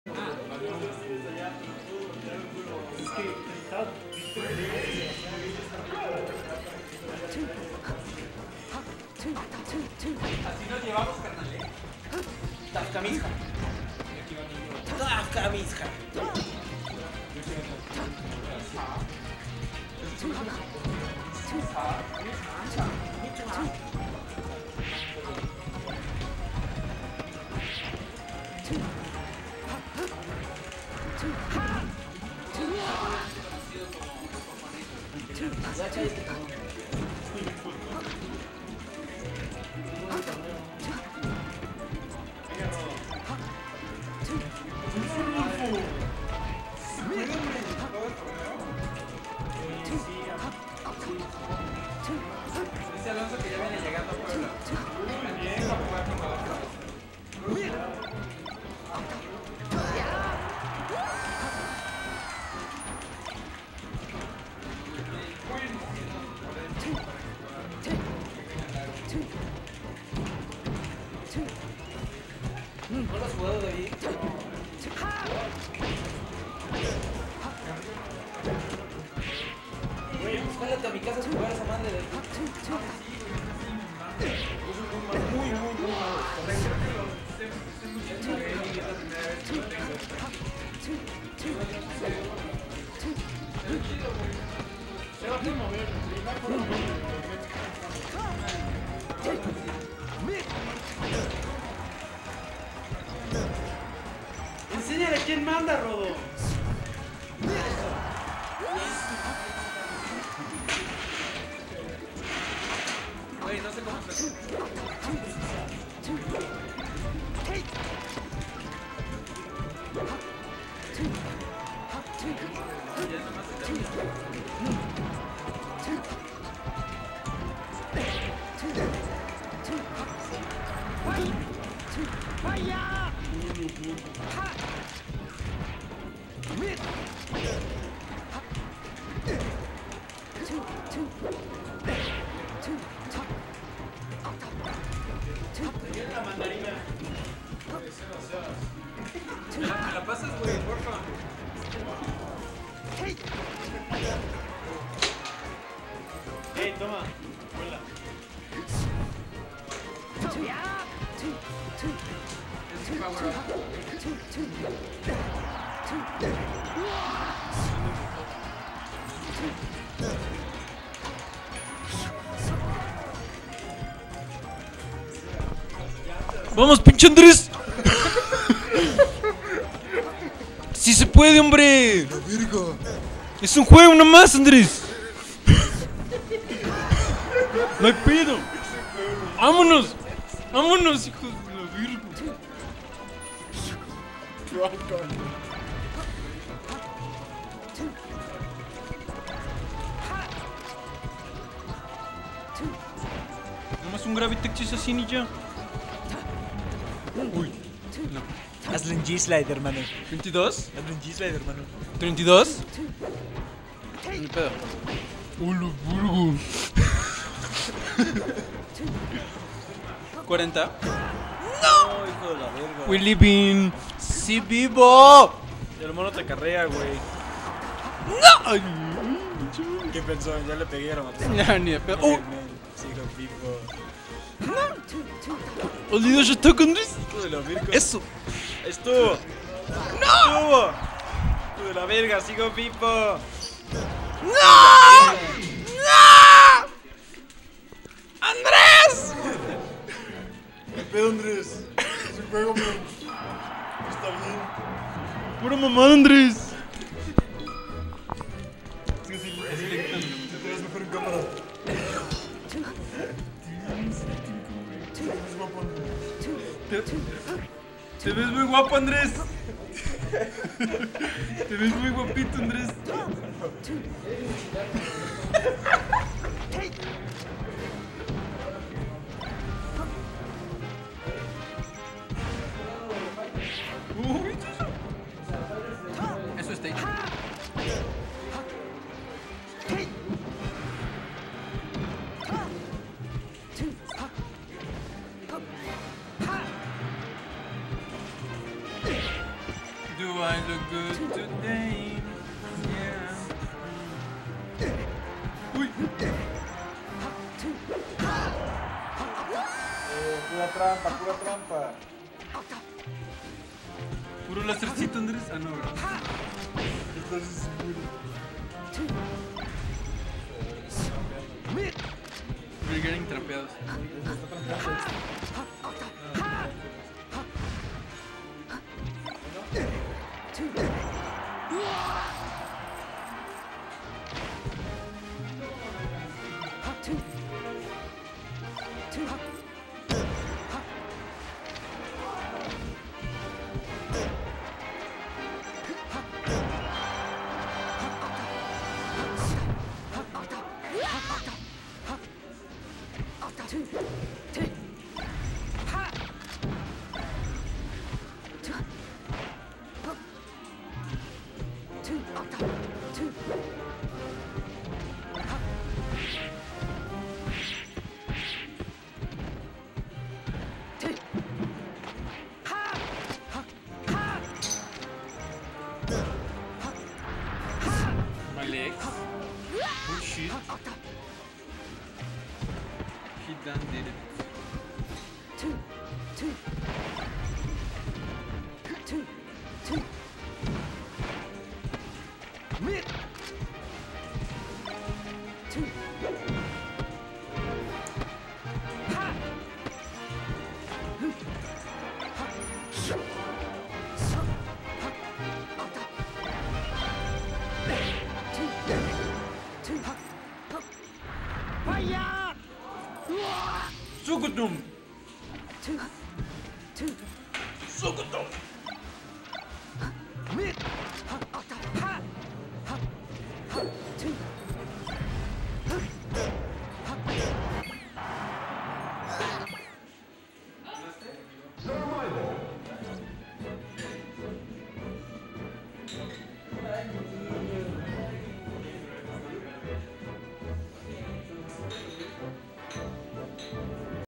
LUF 12 - Malex (Fox) Vs. NG | Epyon (Sheik) - Winners Round 2
Level Up Fridays 12, 23/02/2018, Puebla, Pue.